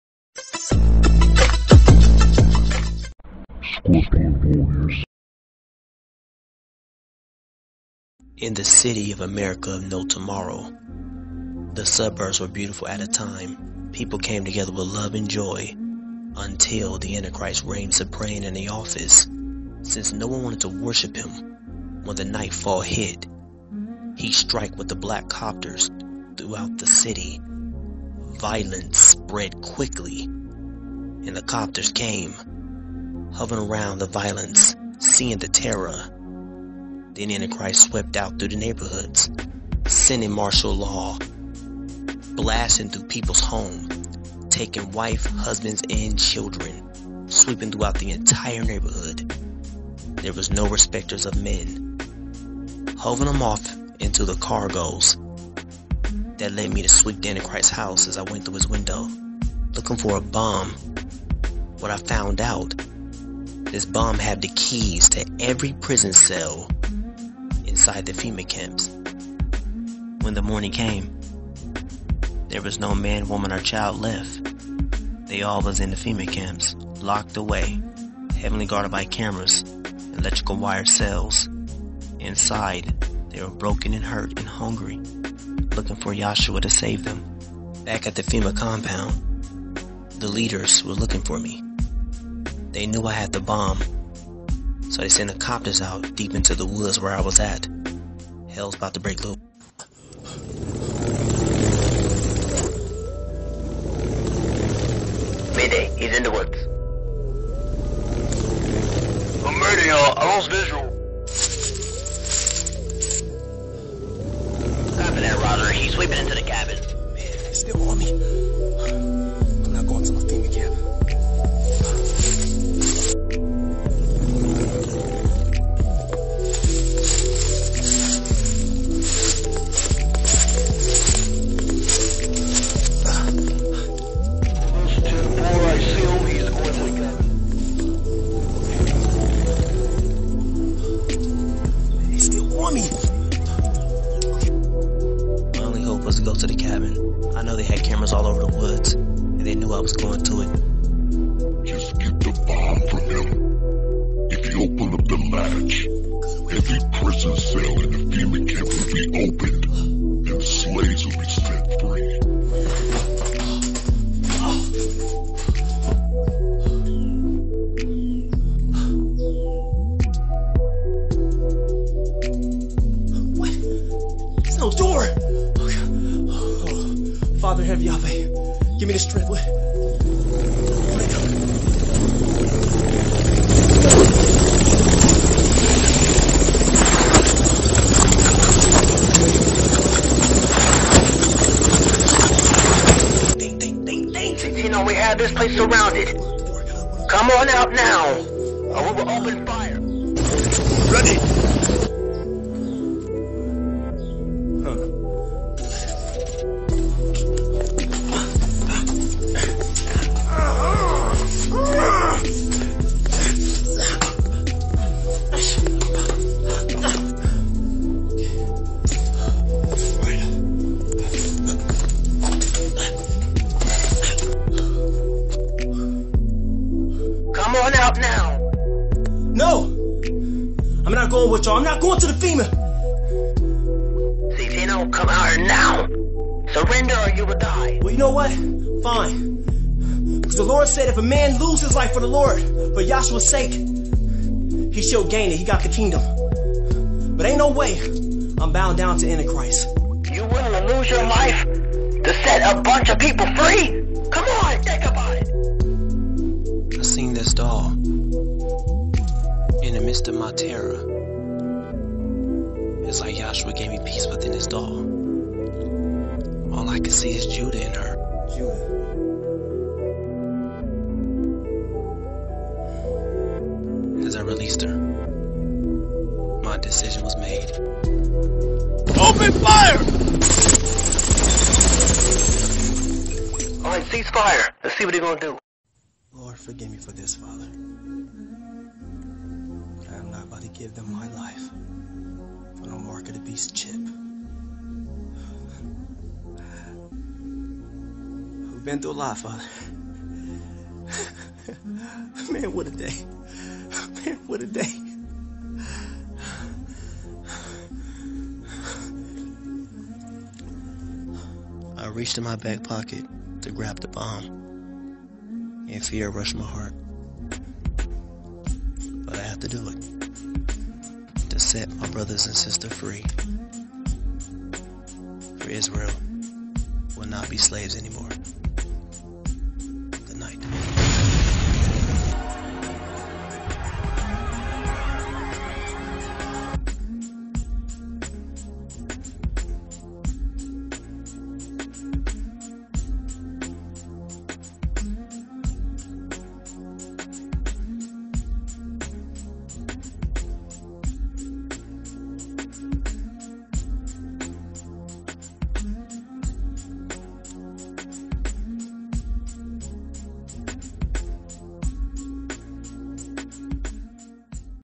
Escaping Fema Movie 2016
This video is about Ctno Rose Escaping Fema as he finds a secret bomb to unlock the other prison sells to let free other slaves that trapped under the Antichrist Hands. It's his life or the slaves dies enjoy